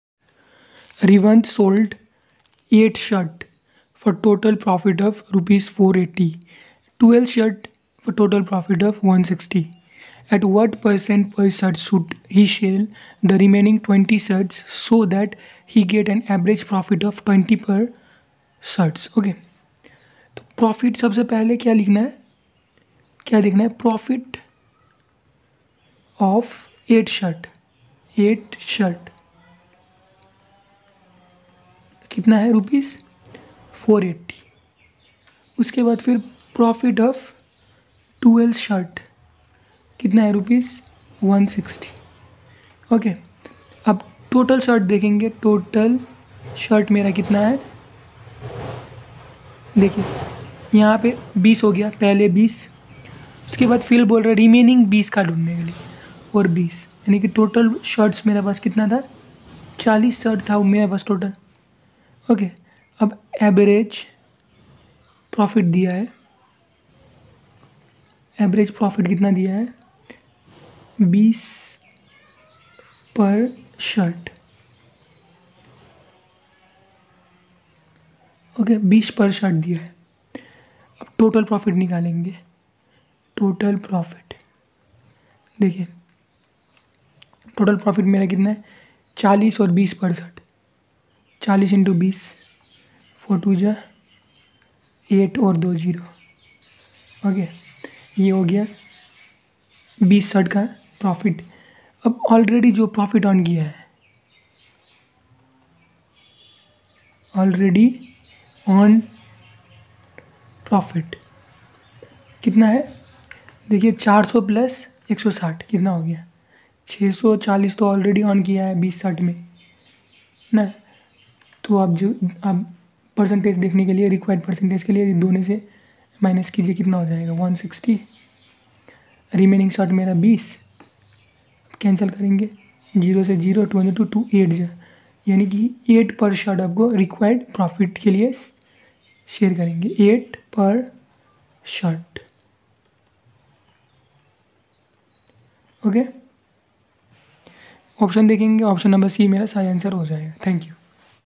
Revanth sold 8 shirts for a total profit of Rs. 480 and 12 shirts for a total profit of Rs. 160. At
Revanth sold 8 shirts for a total profit of Rs. 480 and 12 shirts for a total profit of Rs. 160. At what profit per shirt should he sell the remaining 20 shirts so that he gets an average profit of Rs. 20 per shirt?
रेवंत ने कुल लाभ पर 8 शर्टें बेचीं।  480 और 12 शर्ट पर कुल लाभ रु.  160. उसे शेष 20 शर्ट प्रति शर्ट कितने लाभ पर बेचनी चाहिए ताकि उसे रु. का औसत लाभ प्राप्त हो।  20 प्रति शर्ट?